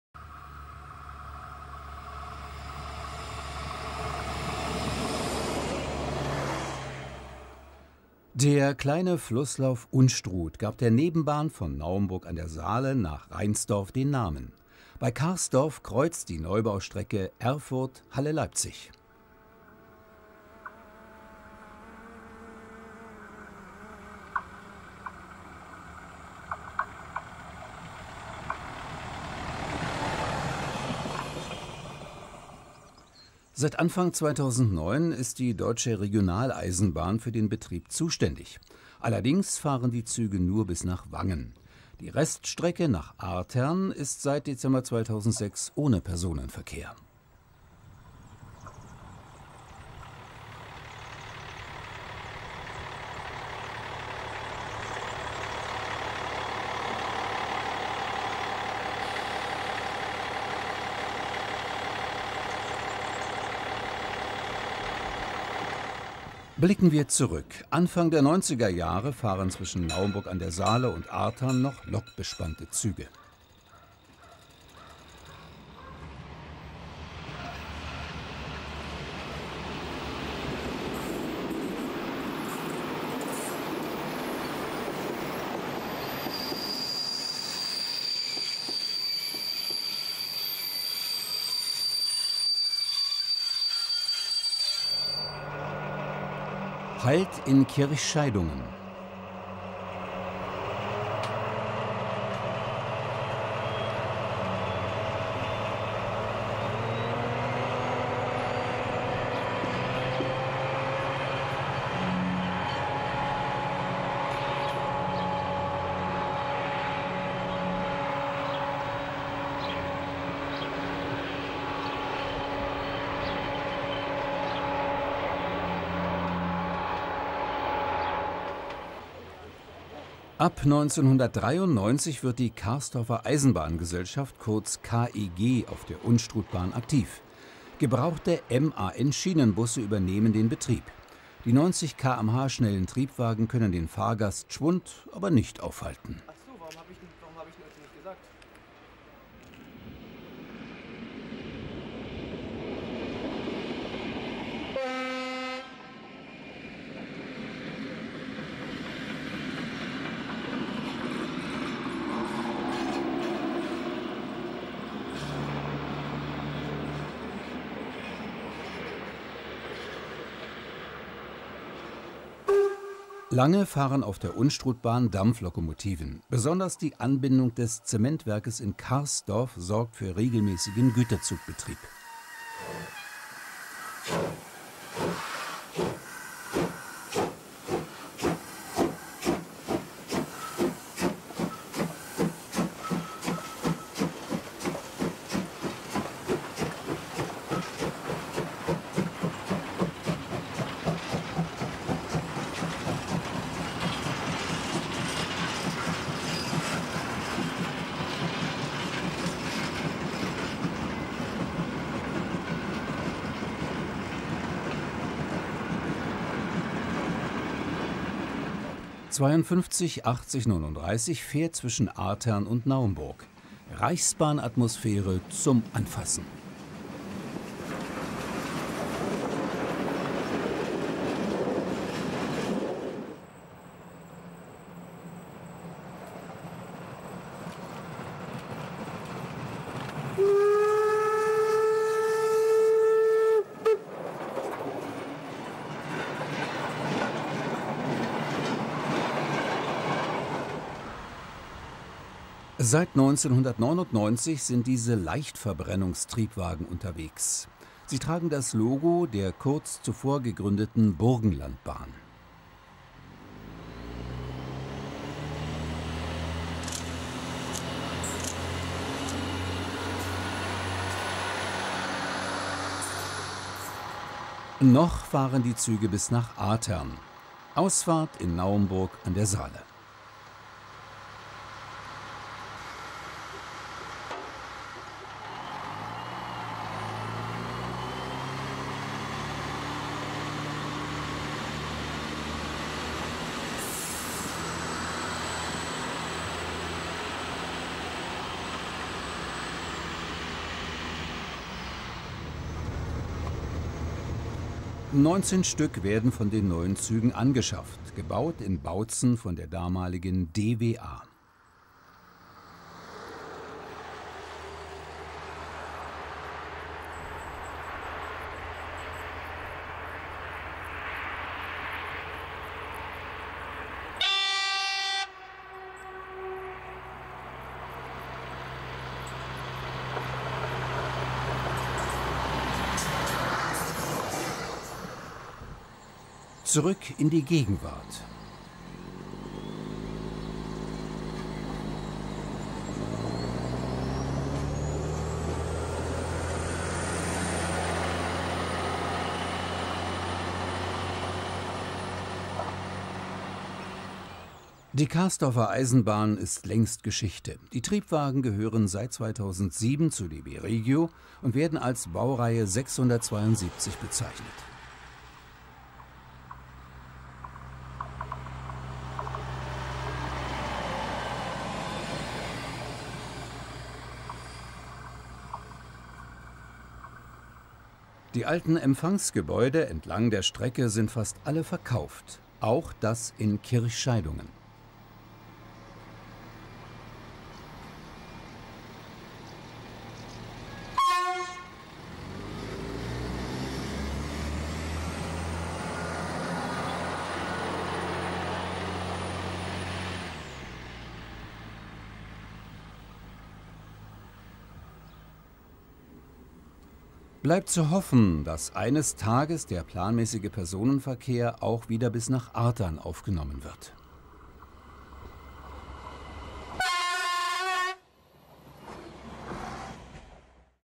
Die Unstrutbahn
Die Bahn zwischen Naumburg und Artern wird Unstrutbahn genannt. Im Laufe der Zeit haben viele Fahrzeuge den Betrieb bestimmt. V100, Ferkeltaxen, MAN-Triebwagen und moderne Leichttriebwagen. Auch Dampfsonderzüge sind von Zeit zu Zeit unterwegs. Wir zeigen Ihnen die Fahrzeuge. Viel Spaß beim Anschauen. Hier mehr
Für nur 2,99/Monat Kanalmitglied werden und inzwischen 90 Filme in voller Länge und ohne Werbung Anschauen.